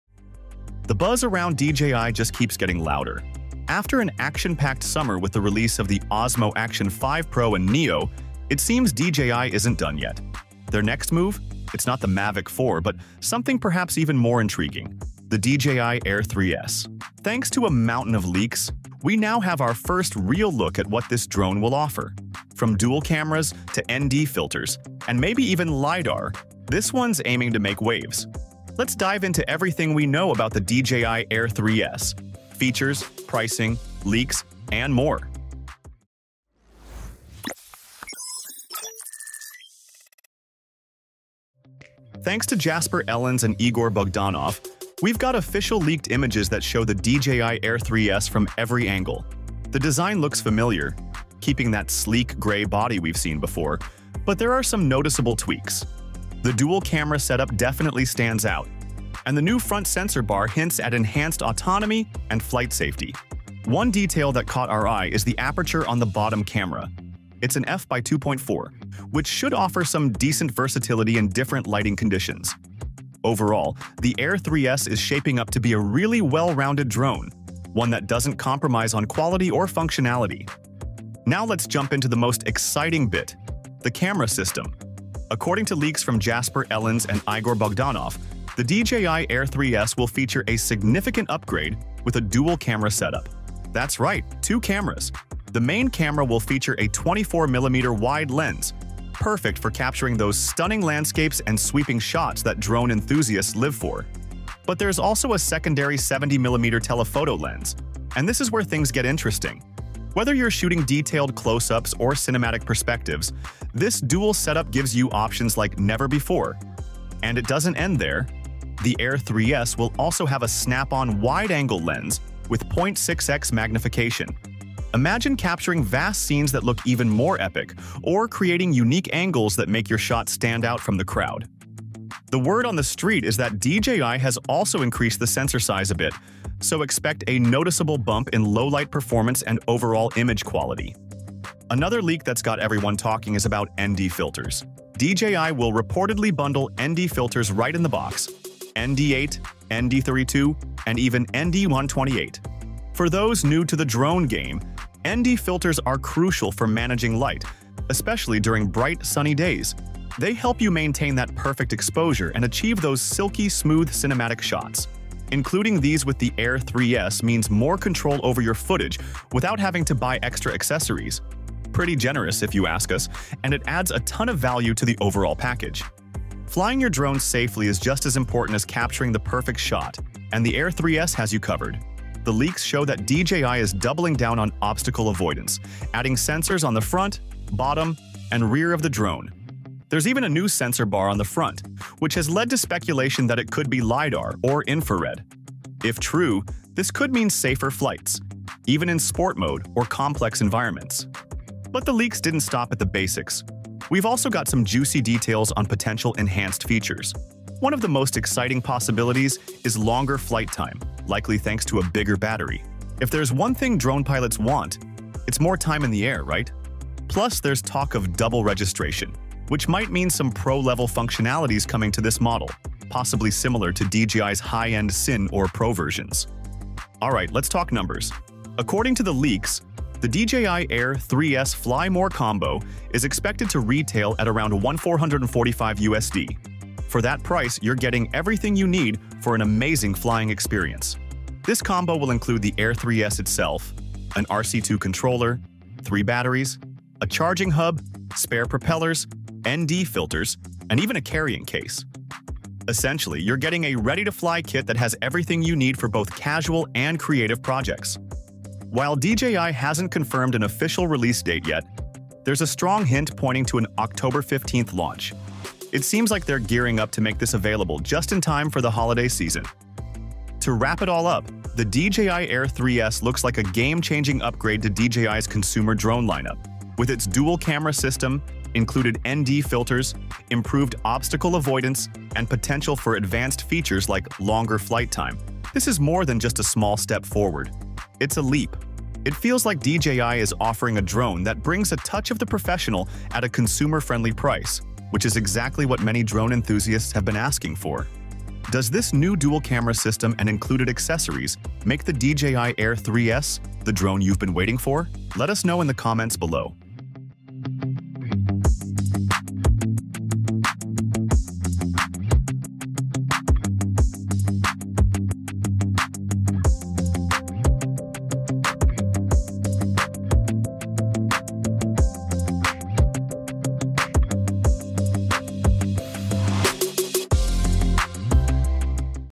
DJI Air 3S Leaks - Coming This October!
DJI is keeping the momentum going after a busy summer, which saw the launch of the Osmo Action 5 Pro and Neo. But they aren’t stopping there. While many were expecting the Mavic 4, DJI has a different surprise in store introducing the DJI Air 3S. Thanks to a slew of leaks, we’ve gotten our first real glimpse of what this drone might bring to the table. From a dual camera system to ND filters, and possibly even LiDAR, the Air 3S is shaping up to be a game changer. Here’s everything we know so far!